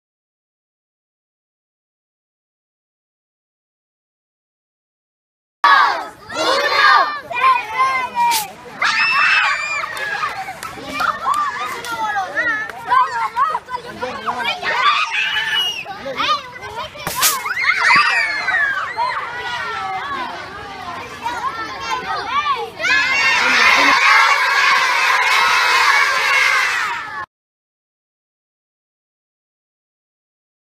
Planetario COSMOS en Valledupar!
Los chicos del valle se la gozaron con el mejor planetario de la COSTA!